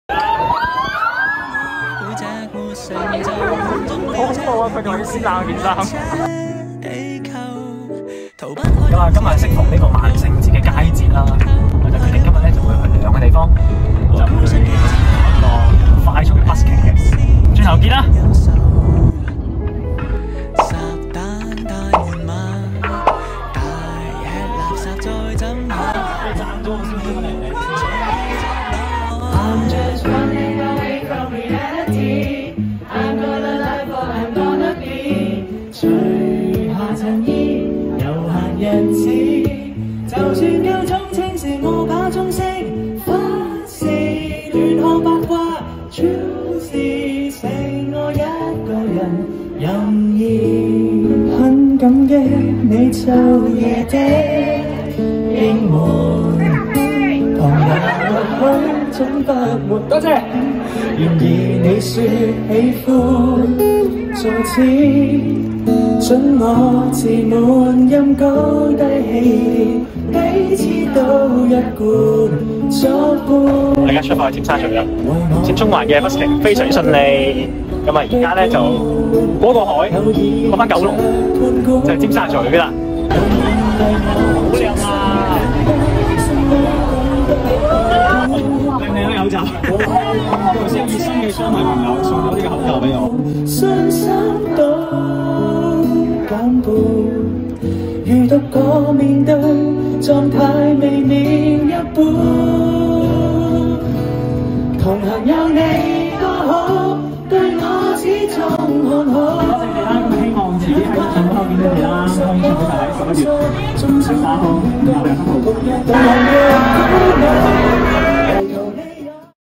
VLOG #3 HALLOWEEN 街頭快閃
人生第一次busking!
同你地一齊大合唱好爽
演唱會再來過 ok?